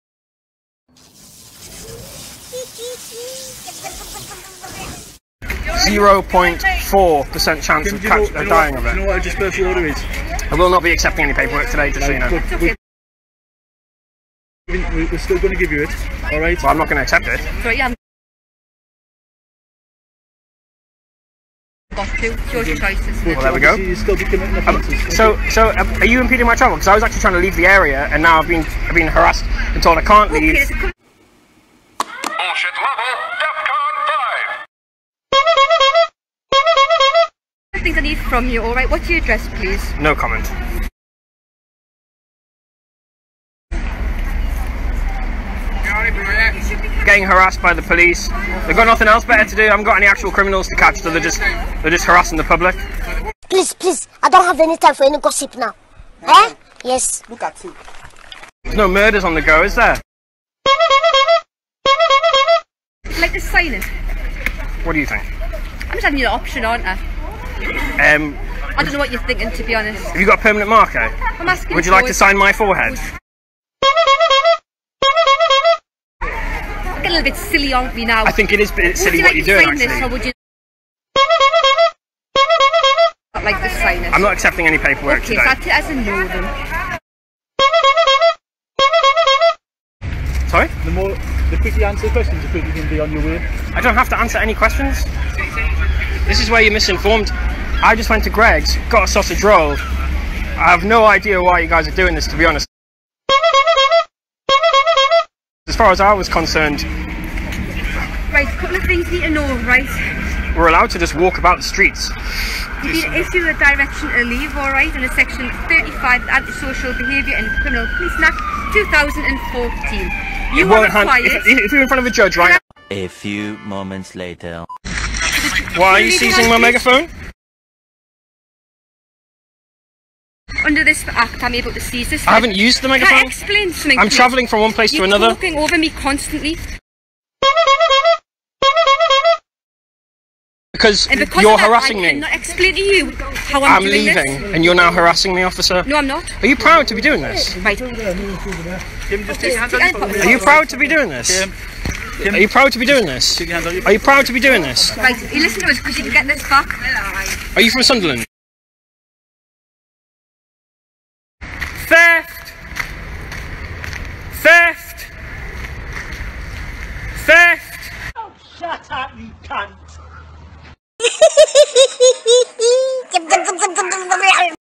Kim Drops Mega Phones are essential for buying a Greggs sausage roll 🙄
What is a Dispersal Order?

Section 35 of the Anti-Social Behaviour Crime and Policing Act 2014 gives a Police Constable and a Police Community Support Officer in uniform the power to exclude a person from an area for a period of up to 48 hours with an Inspector's authority.